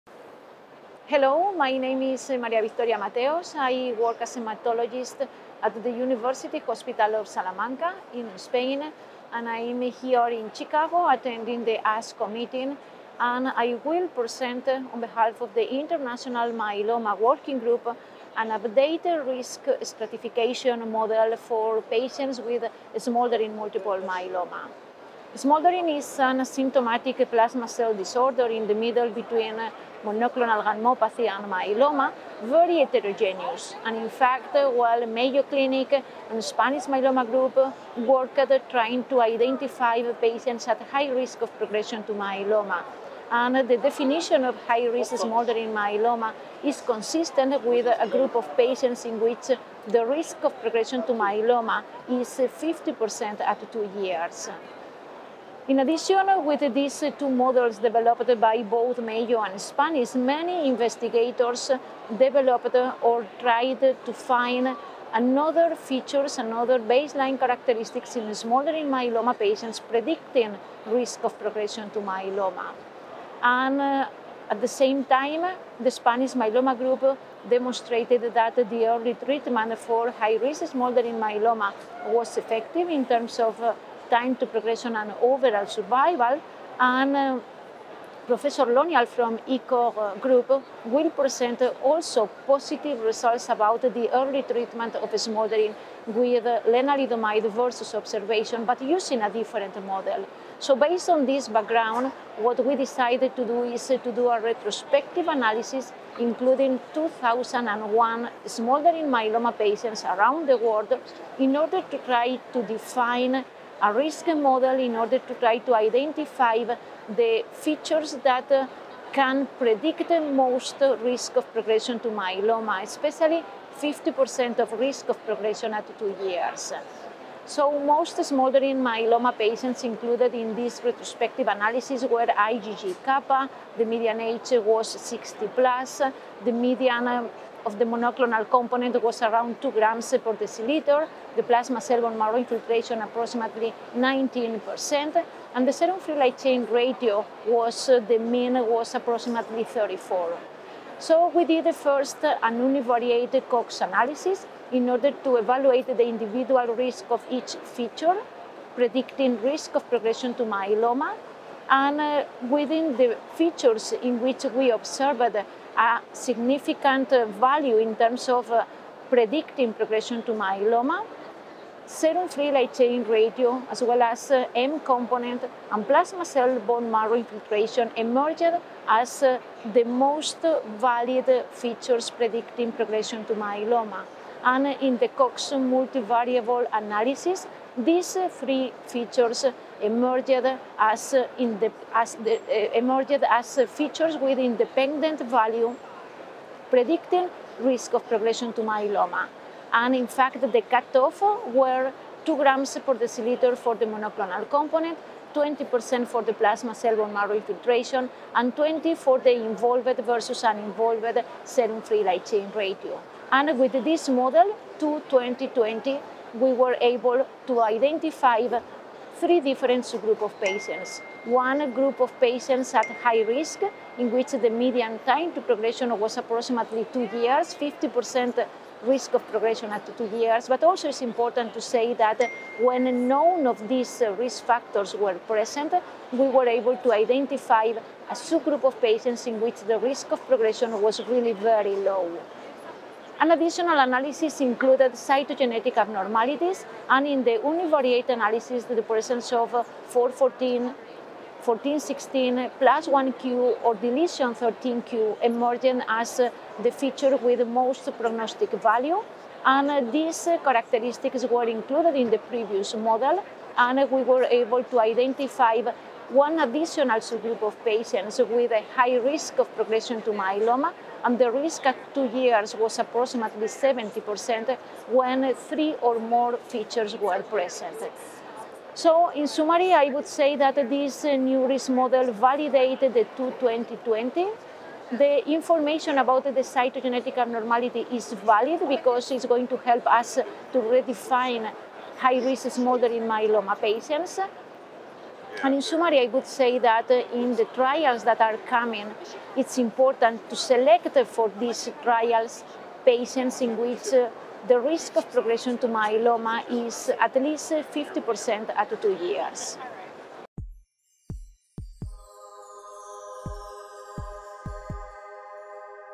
Updated risk stratification model for smoldering multiple myeloma
Updated risk stratification model for smoldering multiple myeloma (SMM) incorporating the revised IMWG diagnostic criteria

Maria-Victoria Mateos, MD PhD
University Hospital of Salamanca
Salamanca, Spain

Background:

The diagnostic criteria for MM were revised in 2014, recategorizing ultra-high risk (i.e., 80% at two years) as active myeloma requiring therapy. The removal of patients at the highest risk of progression from the smoldering group requires reassessment
of current risk stratification models.

Conclusions:

We have developed a risk stratification model for SMM that incorporates revised cutoffs for previously used parameters (20/2/20) that can be universally applied. Additional analysis are being conducted to develop models that utilize common cytogenetic abnormalities, as well as those without FLC given lack of availability of all tests across the world.

Category
Nonprofits & Activism
License